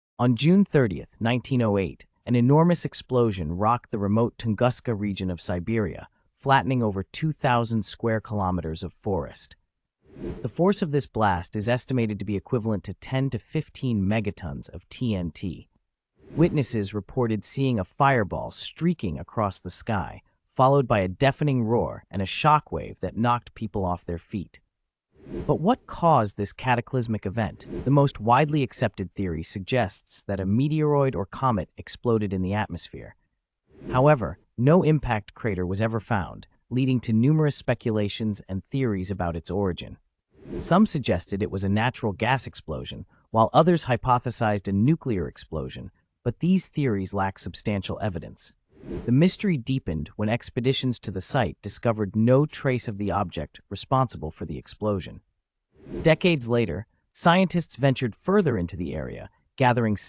The Tunguska Mystery Unveiled
What caused the massive explosion in Siberia in 1908? Dive into the enigma of the Tunguska Event. tunguska history MysteriousEvents siberianhusky explosions science